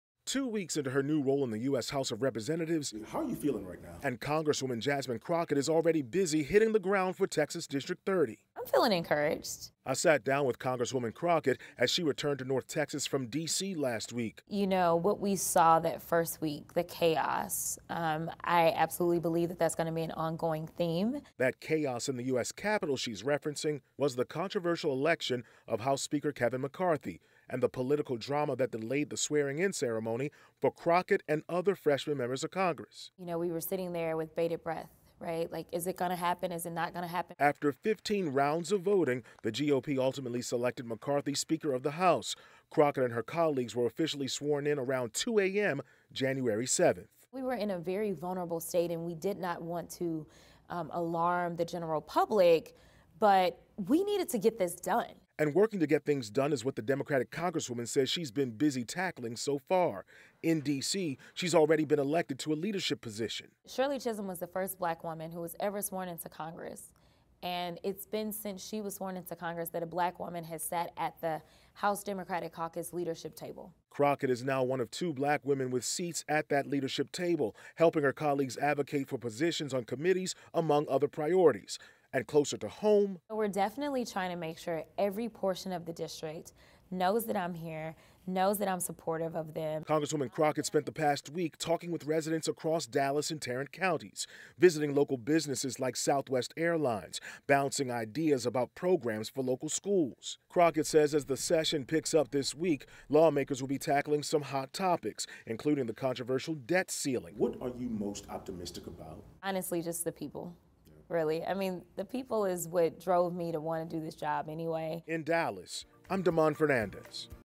Dallas-area congresswoman Jasmine Crockett outlines her plans in Congress
U.S. Rep. Jasmine Crockett represents District 30, which had been represented by retired congresswoman Eddie Bernice Johnson for over 30 years.